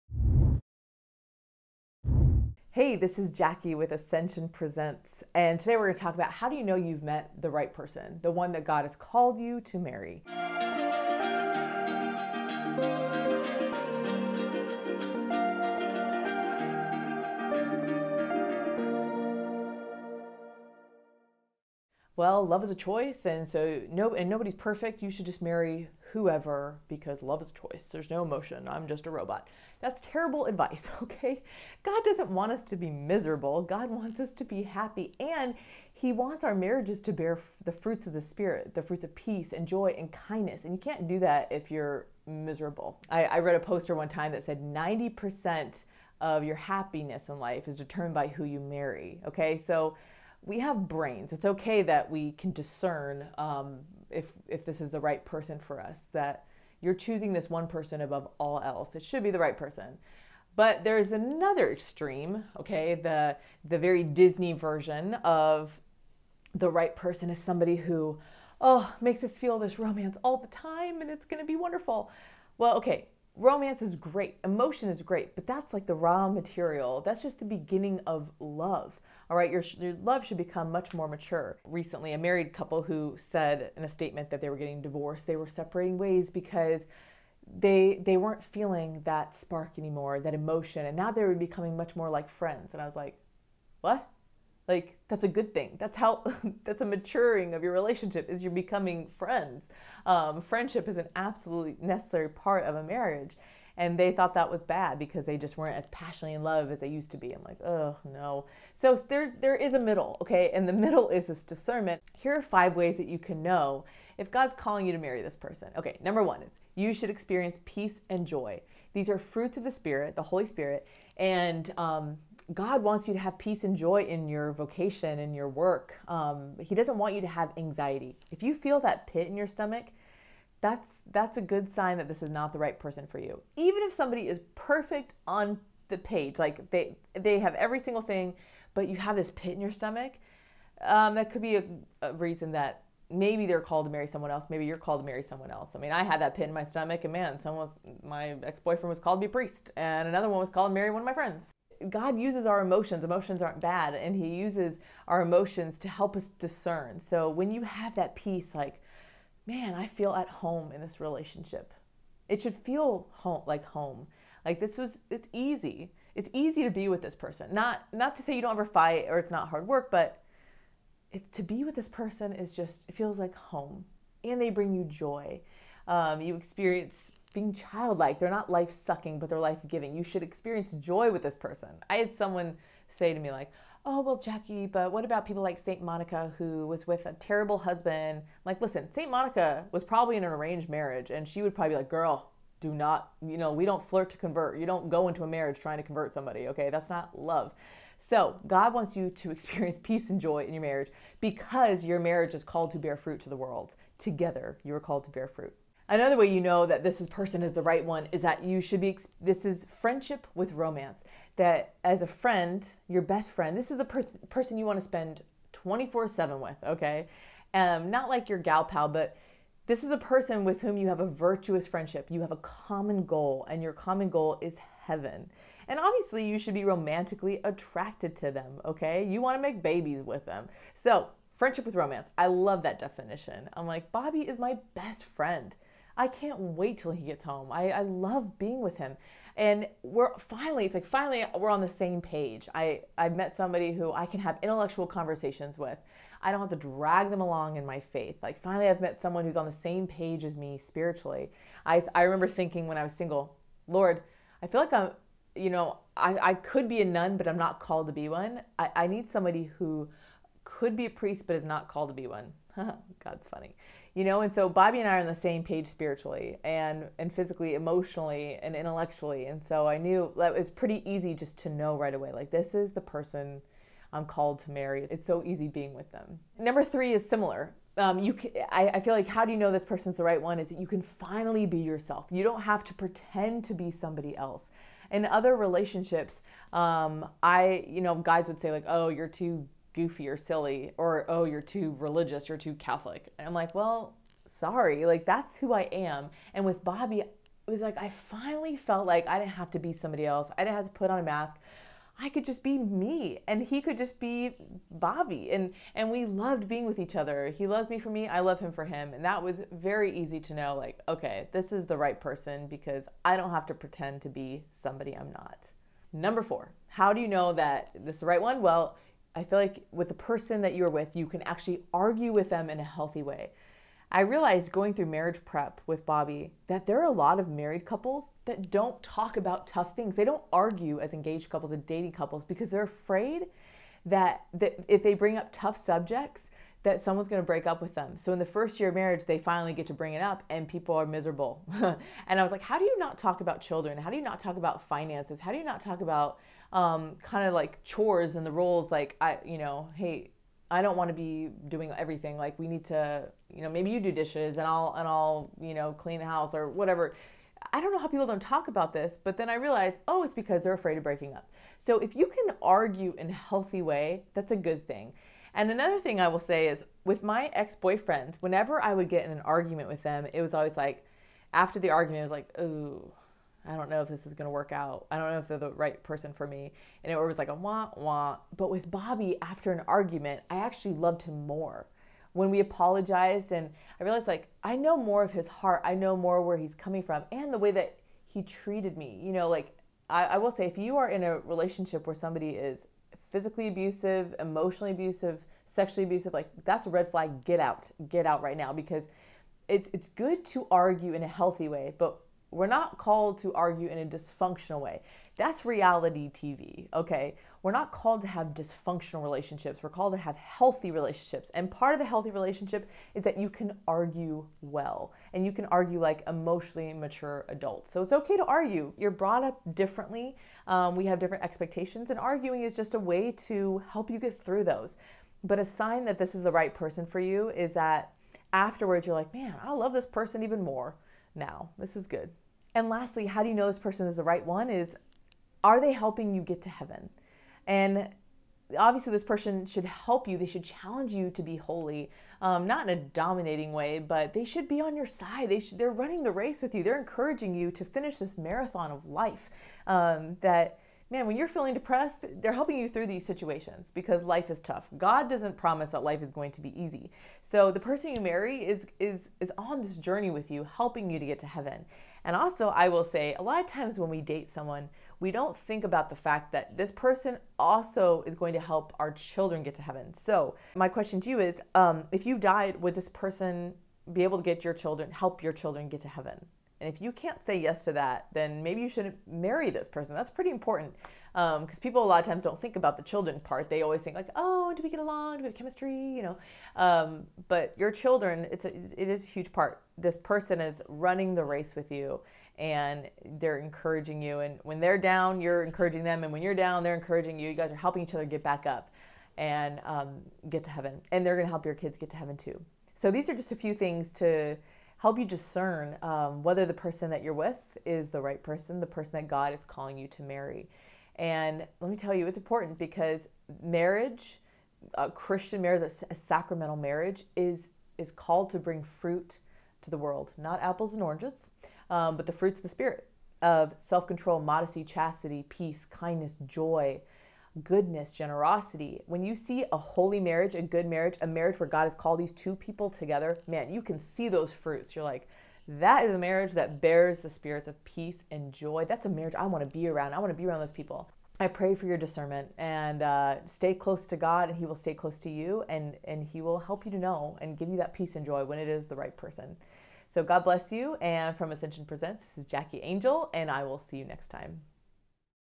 How to Know if You've Found "The One"
Staying on the straight and narrow path can be difficult when you’re discerning marriage and looking for “the one”.

Jackie says we live in a culture where people either say “Love is a choice. Marry whoever,” or they say that you should feel the romance all of the time when you’re with them.

There is a middle ground between these two extremes, and Jackie helps us find it in this video.

She says there are five things you should look for when deciding who to marry. These five things are good signs that he or she is “the one”, meaning the one who will help you and your children get to heaven:

1. You experience peace and joy in the relationship.
2. You have a common goal, which is heaven.
3. You can finally be yourself around them.
4. You can argue with him or her in a healthy way.
5. You’re confident he or she will help you and your children get to heaven.

In other words, don’t get caught up in the romantic, but don’t entirely ignore it either. If heaven is the ultimate goal, everything else will fall into place.

“But seek first his kingdom and his righteousness, and all these things shall be yours as well” (Matthew 6:33).

Also, make sure you check out Ascension’s Joy-Filled Marriage program, which offers many great life skills for those discerning marriage and married couples.